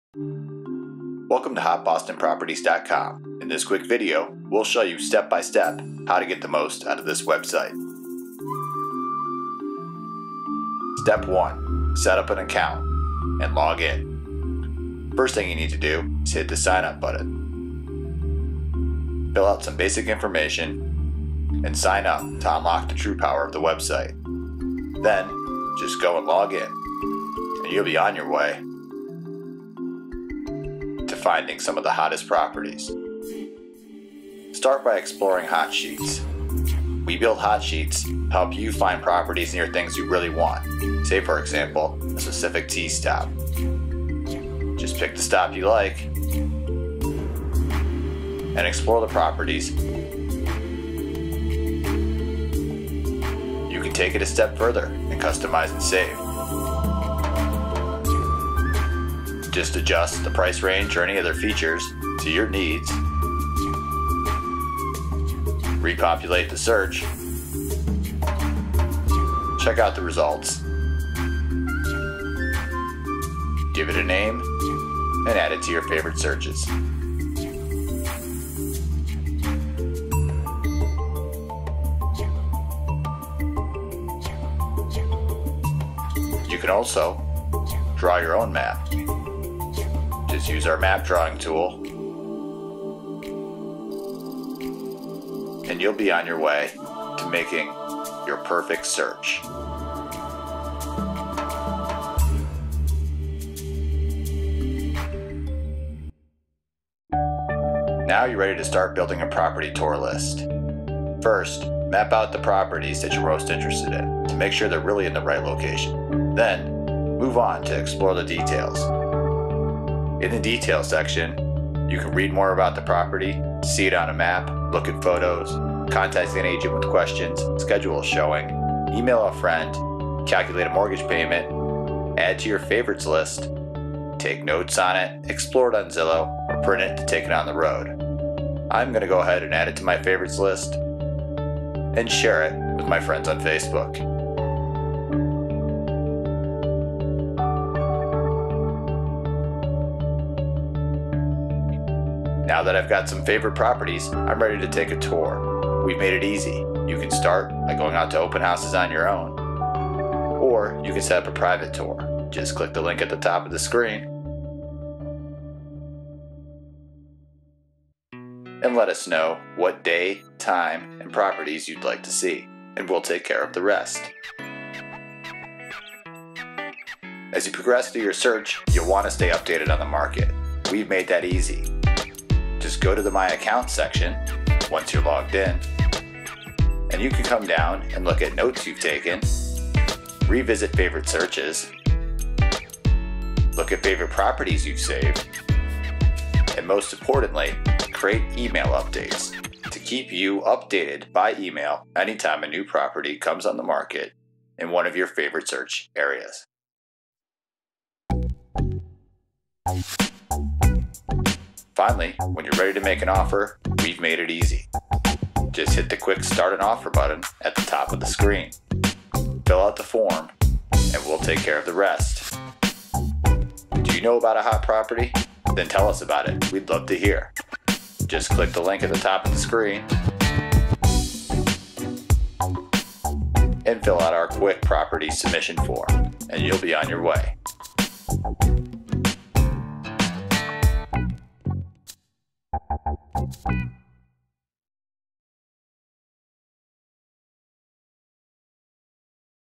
Hot Boston Properties: How To Site Guide
A quick overview of Boston's most powerful property website.  Helping you master the art of quickly finding, touring & learning about Boston's hottest properties!